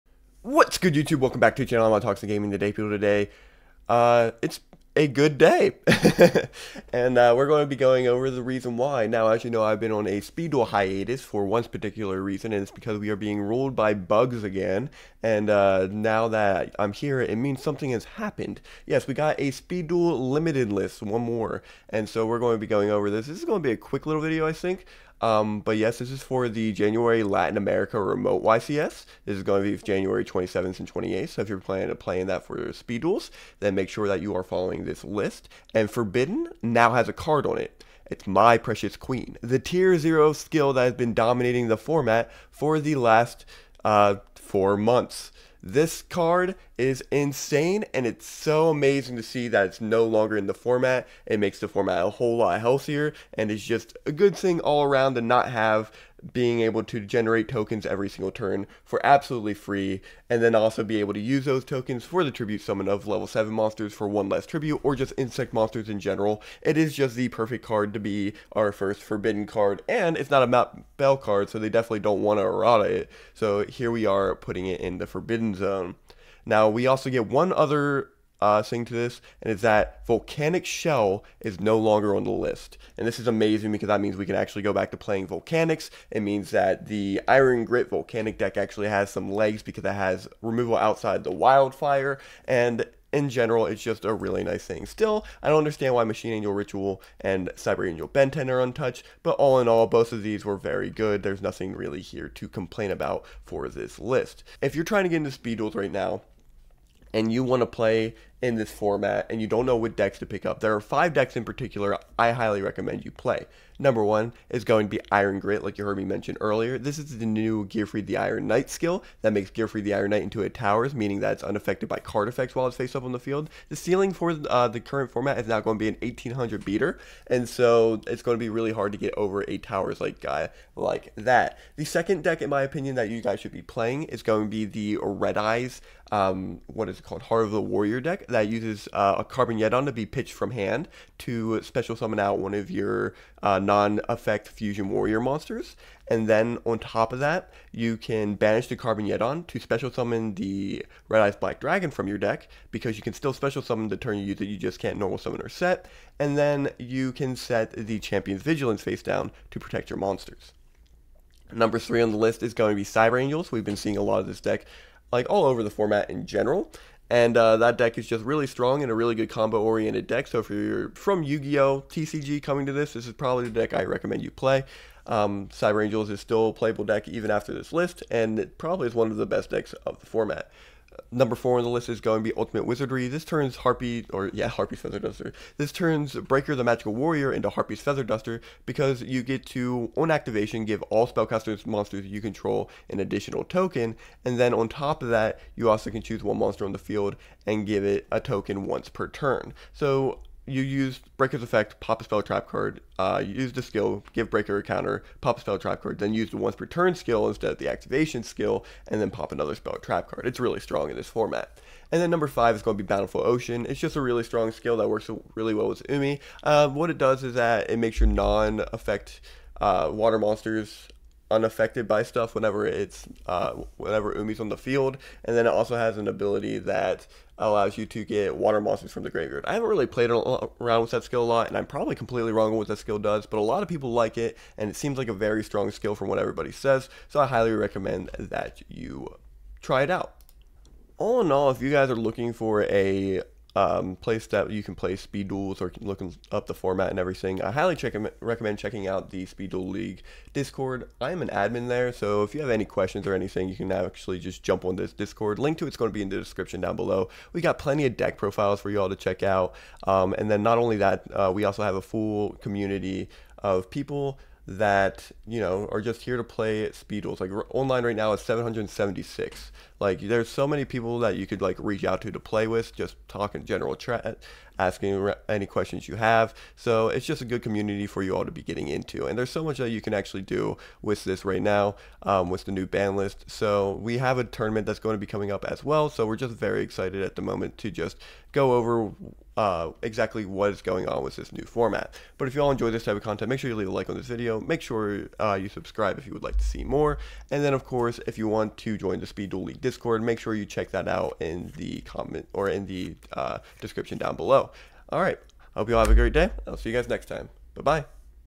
The New Speed Duel Banlist Is FInally Here
Hey everyone MPQ is dead and the format is saved!!! Lets make some content and have some fun :D